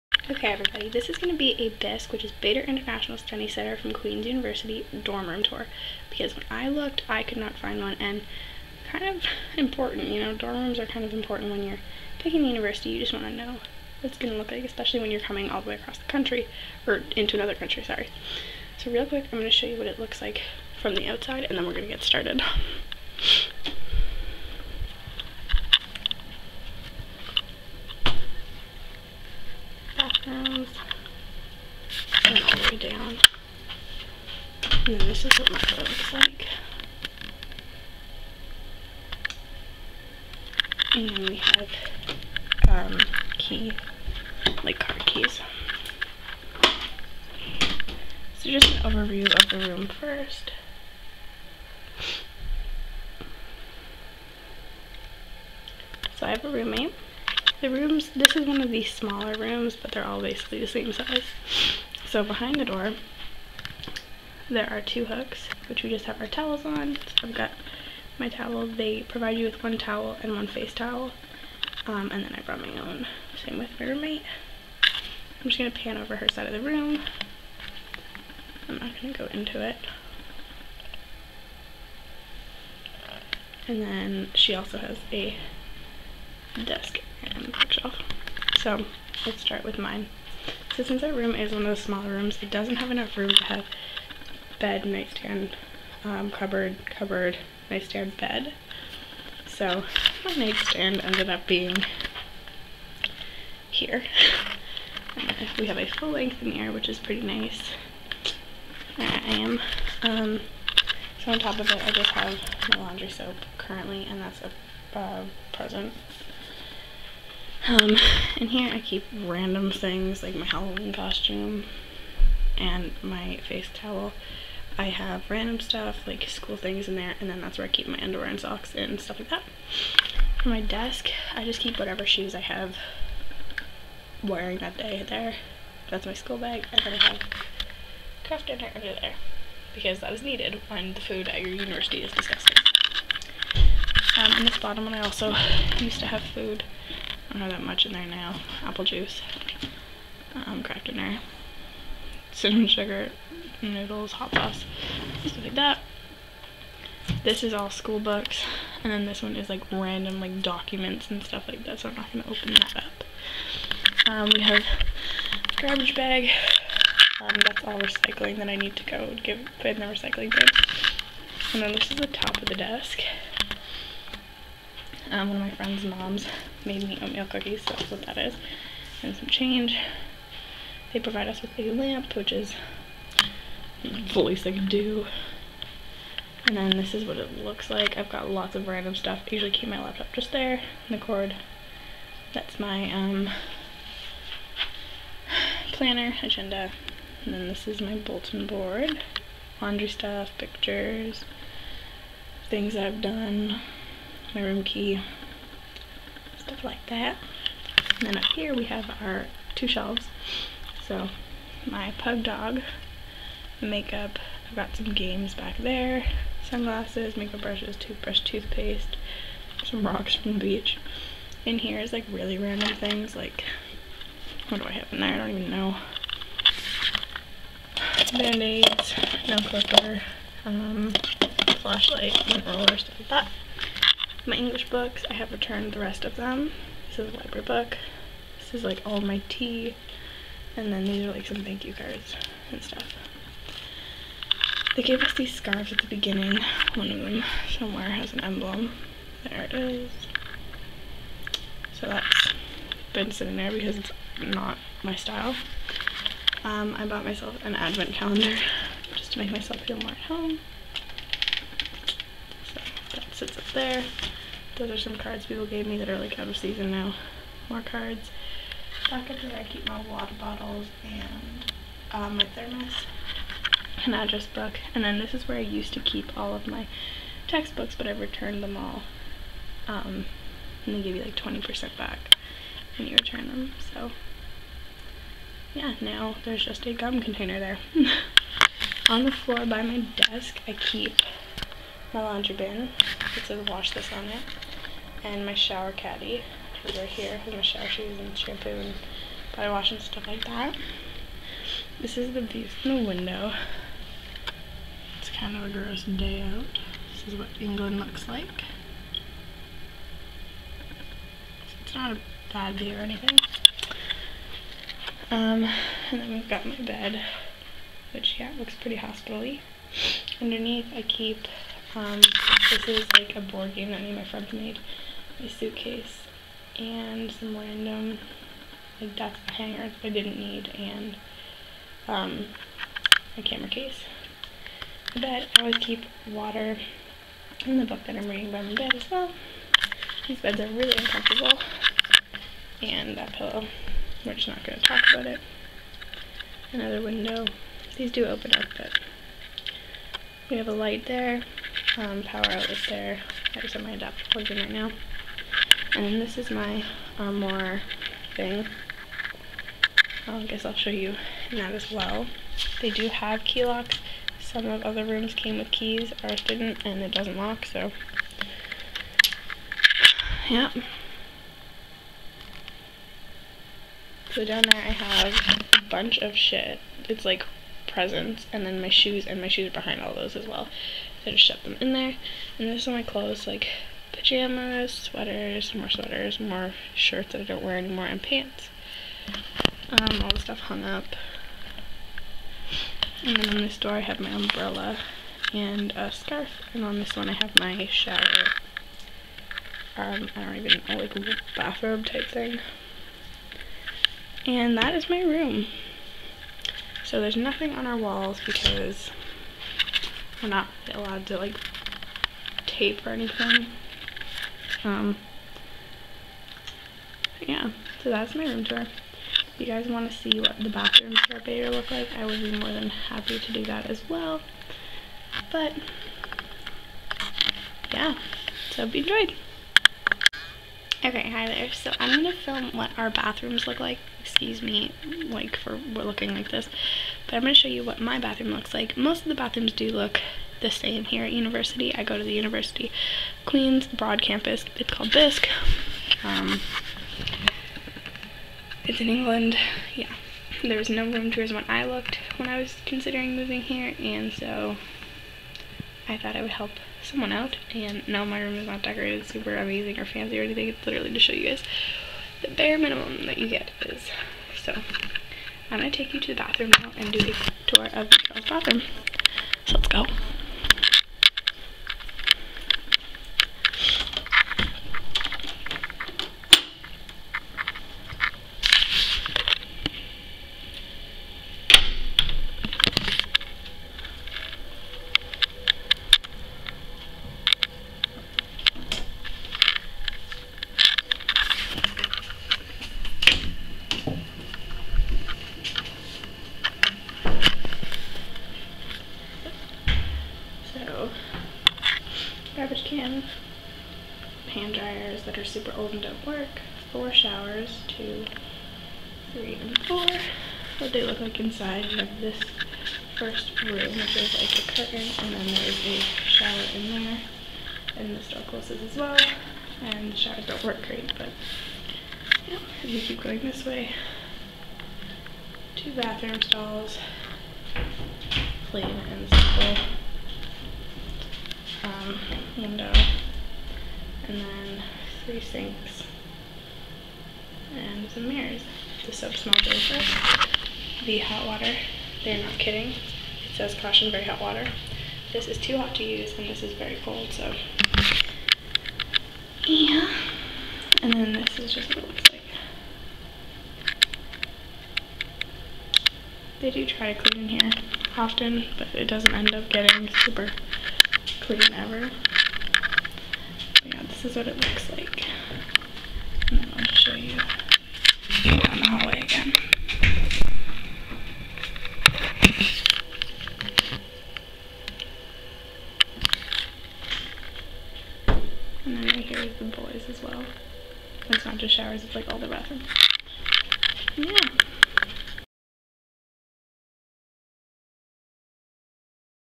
Queens BISC room tour
It was tough picking a university when I could find very little to no information on the actual place itself. If you're a future BISC student I hope this helps you out a little bit and enjoy your time at the castle.